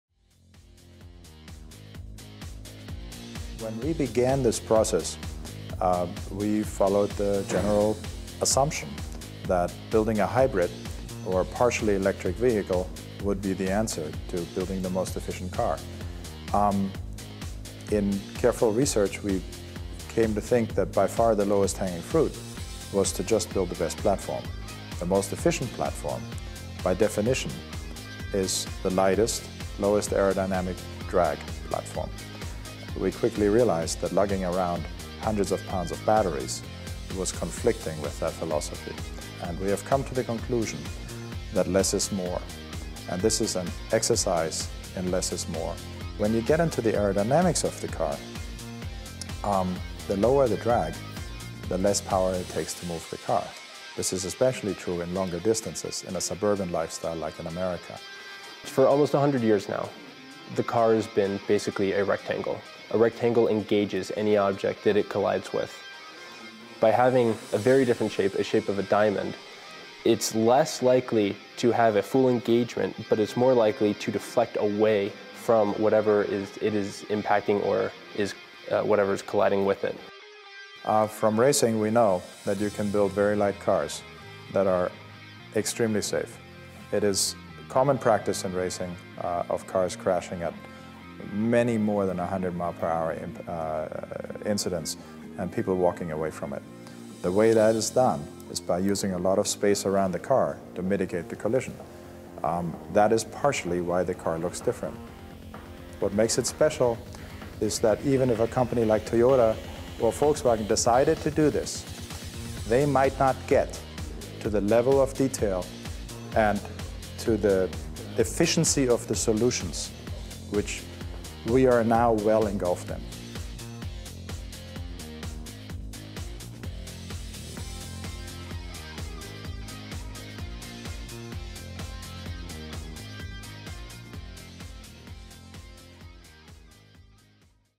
Edison2 The Very Light Car
The Edison 2 X prize team briefly explains some of the virtues of their "Very Light Car."